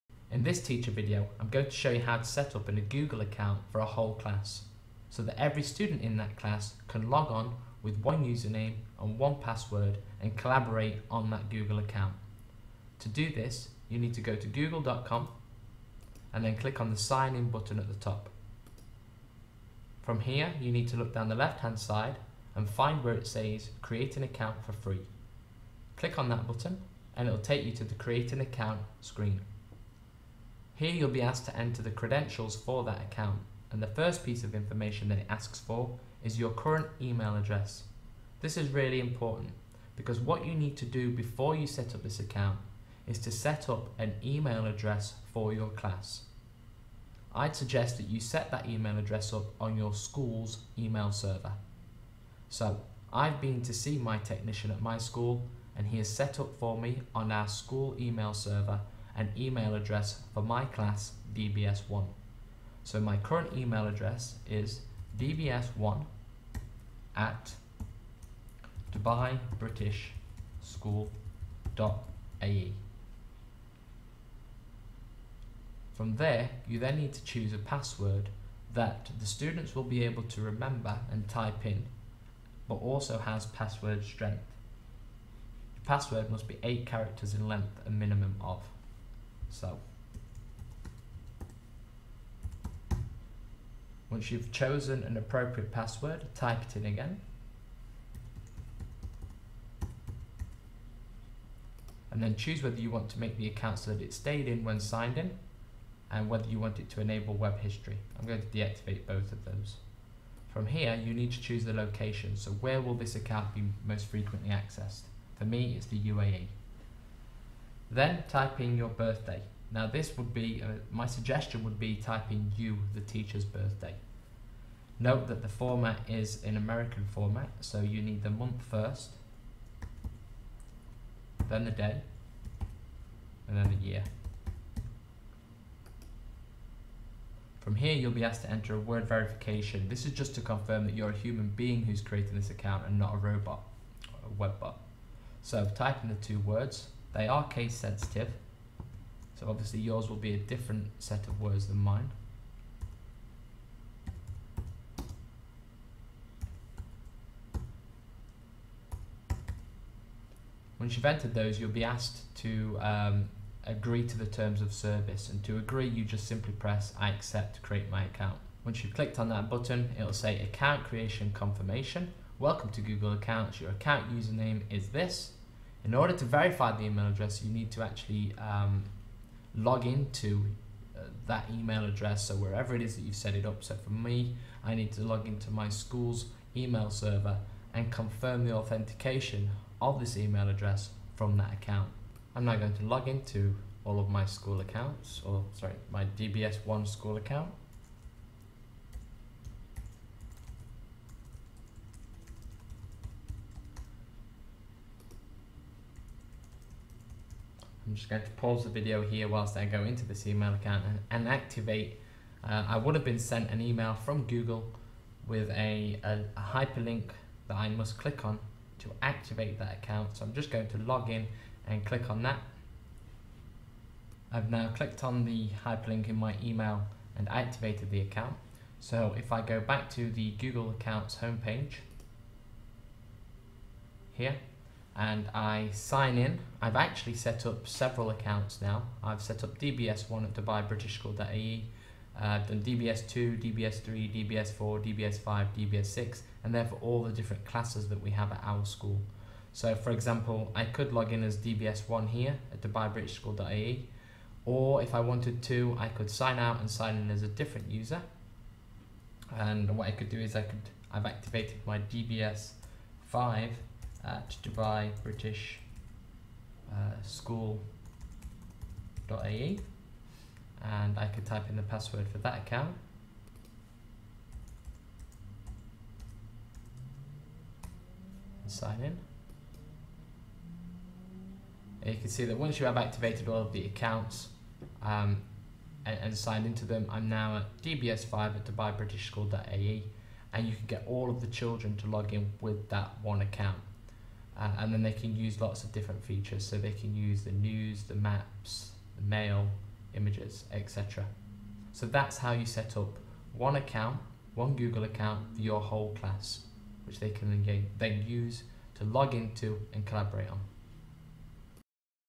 Teacher Video - Setting up a Class Google Account
Demonstrates how to set up a Google Docs class account to find out more)